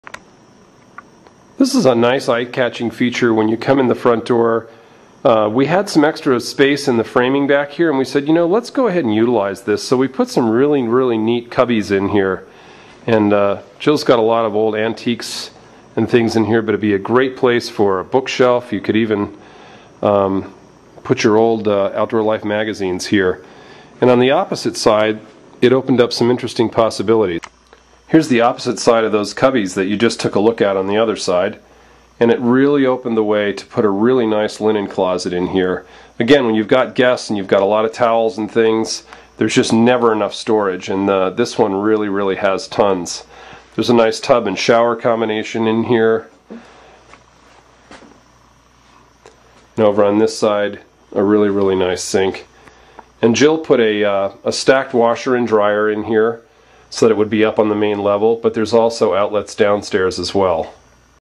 Outdoor Life Dream Cabin Walkthrough: Bathroom and Linen Closet
Outdoor Life went about constructing the perfect dream cabin a year ago.  Thanks to feedback from fellow hunters and fishermen, the dream cabin is now complete.  Located in Cabin on the Hill in Carbondale, Ill.  Editor-in-Chief Todd Smith takes a look at the bathroom on the first floor.  There is a shower/tub combo and a washer and dryer.  The space right outside the bathroom has been transformed into a linen closet to provide more storage space.